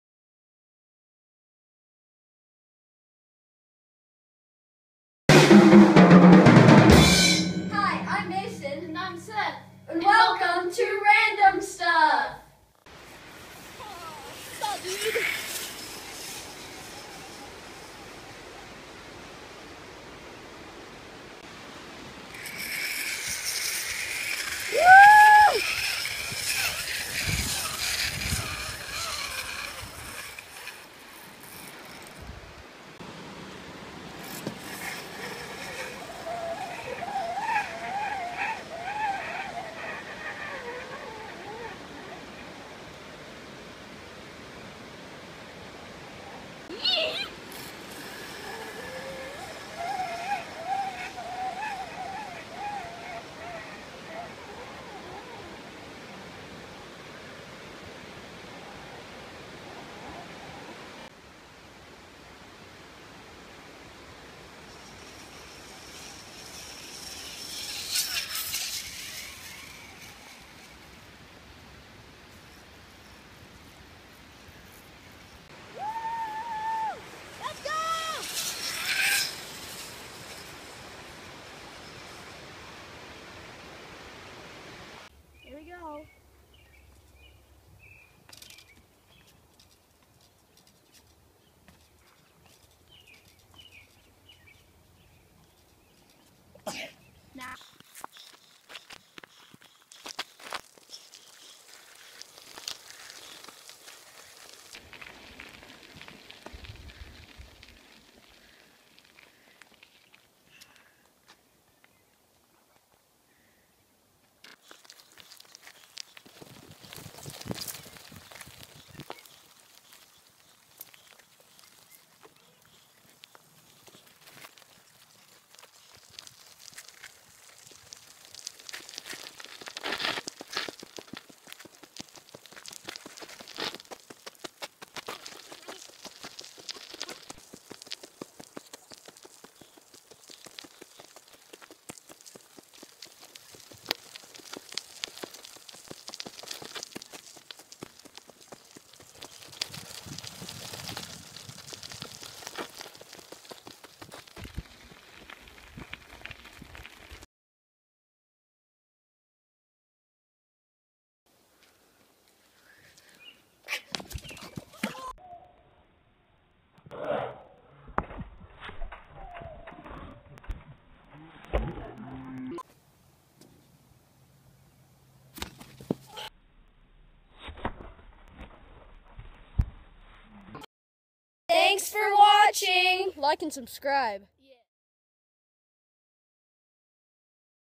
Random Stuff
Mason, Seth, and Skyler filming themselves zip lining, slack lining, and bike riding after the long covid separation (5/29/2020).